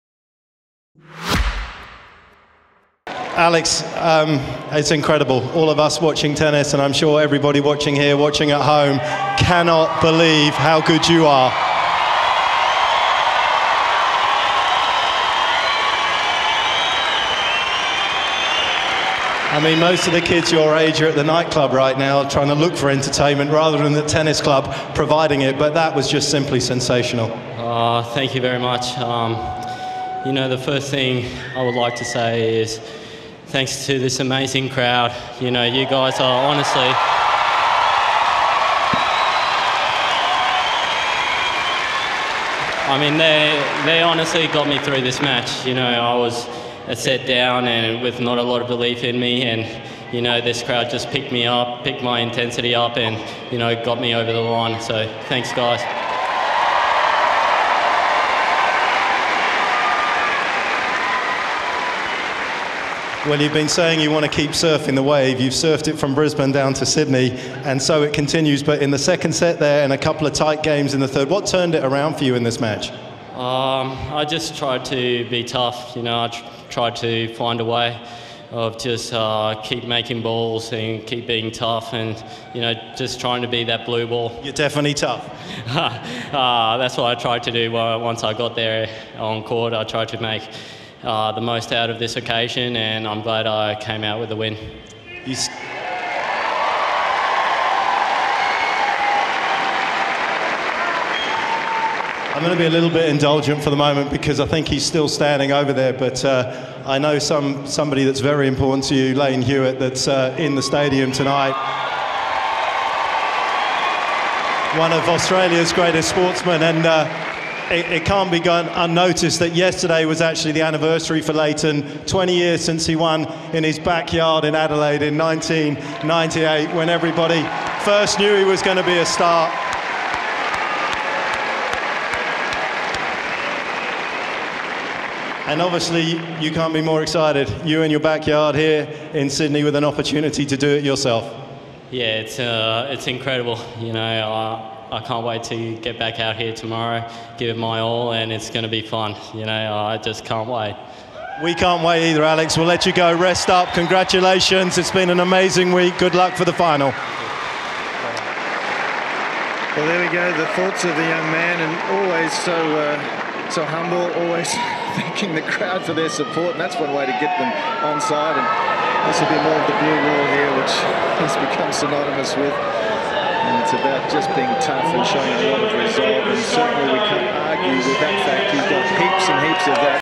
Alex De Minaur On Court Interview (SF) | Sydney International 2018
Alex De Minaur's thanks the crowd during his on court interview after his semi final win in Sydney.